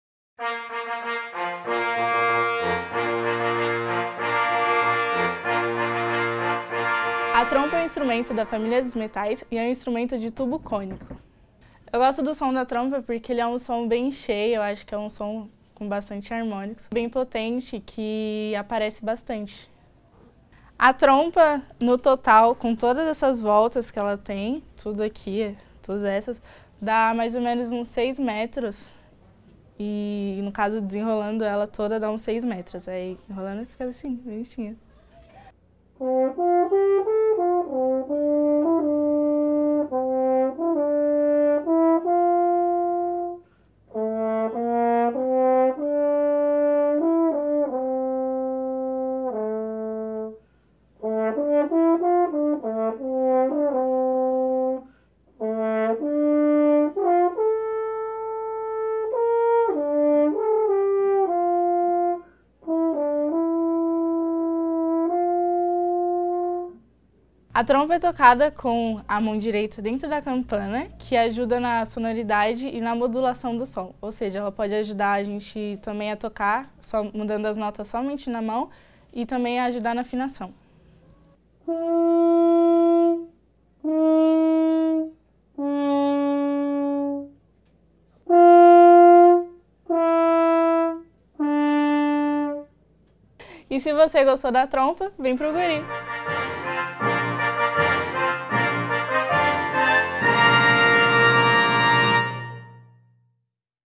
Conheça os instrumentos | Trompa
Nossa aluna Camila Vitótia apresenta a trompa – instrumento da família dos metais.

E aí, gostou? Acesse e confira os polos de ensino que oferecem aulas do instrumento!